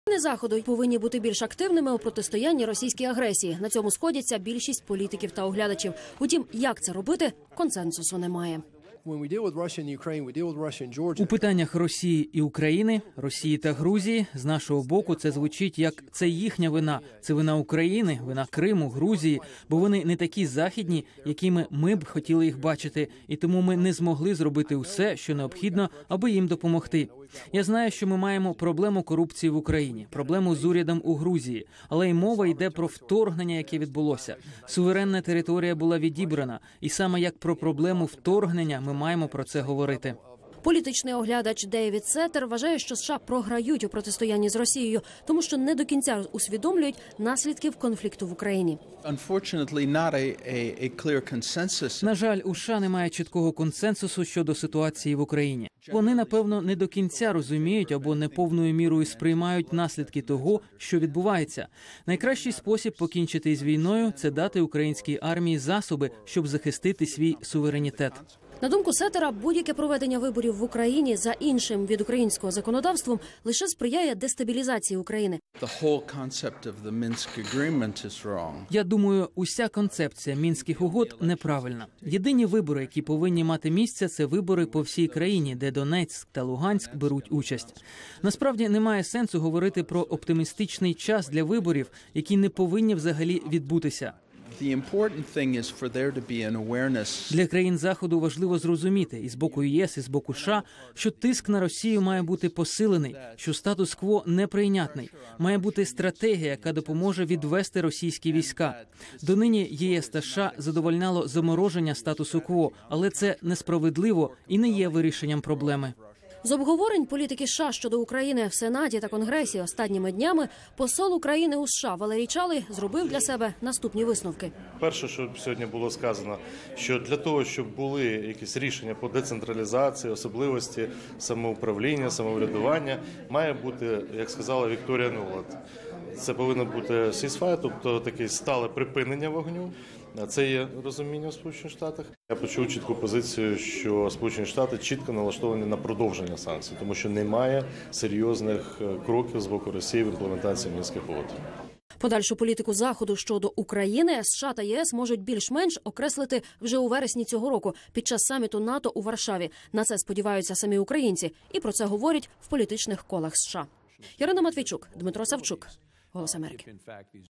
Ось чому у США досі шукають вірний підхід до України, Росії. Відео
Дейвид Сеттер вважає, що США програють у протистоянні з Росією, тому що не до кінця усвідомлюють наслідків конфлікту в Україні.